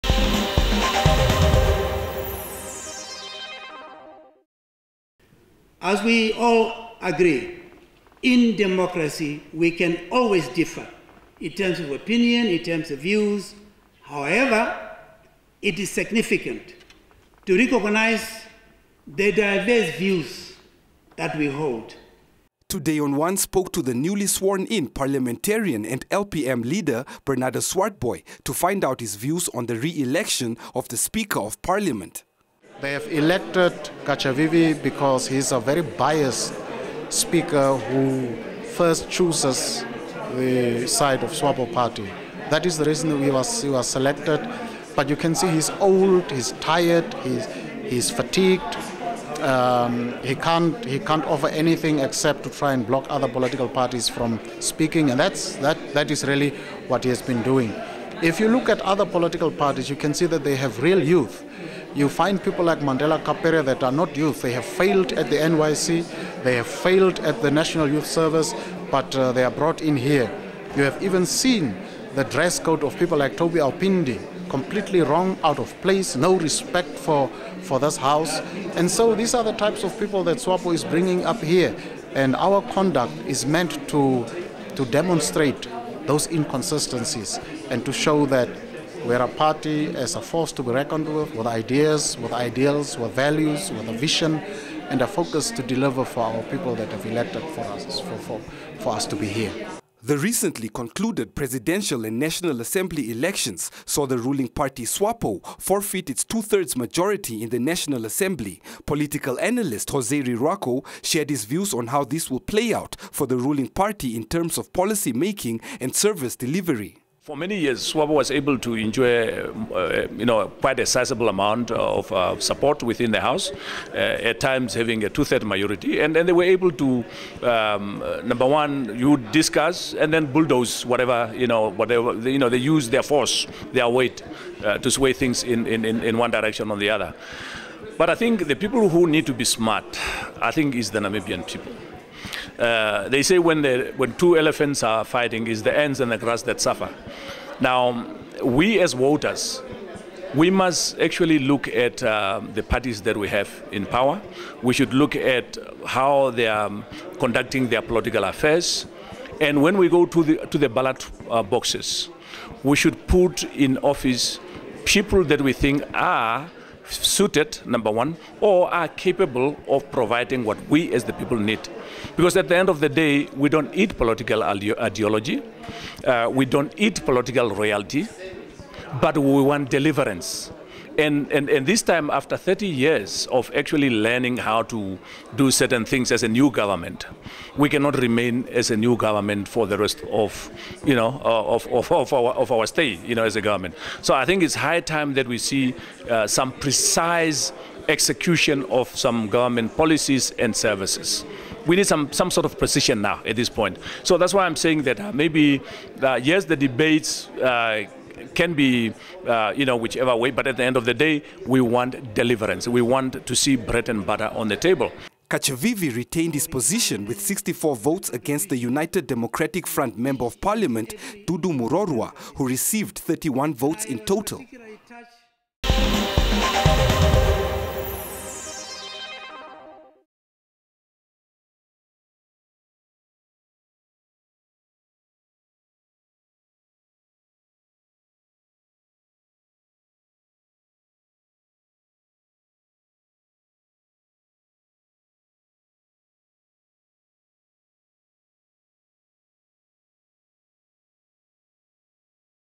Swartbooi criticizes Katjavivi's re-election as Speaker of parliament
The Leader of the Landless People's Movement - Political Party(LPM) has criticized the SWAPO member of Parliament, Peter Katjavivi, who was re-elected as the Speaker of the National Assembly, at the swearing in ceremony of the seventh parliament.

Watch TodayOnOne on DStv 284, GOtv Namibia 90, DStvNow App and tv2africa.com weekdays at 19h00.